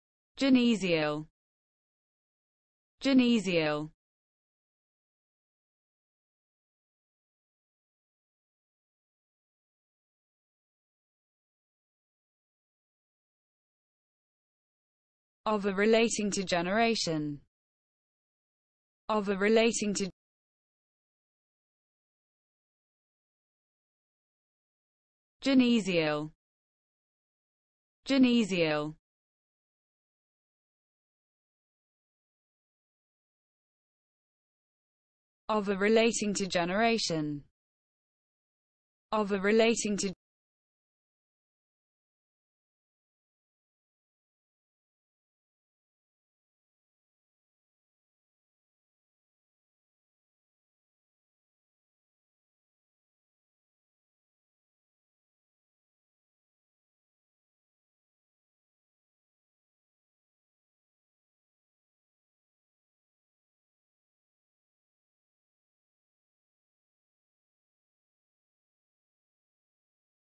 What does Genesial mean?
What does Genesial mean in English? Learn the meaning of the word Genesial! How to Pronounce Genesial. Expand your vocabulary, learn words.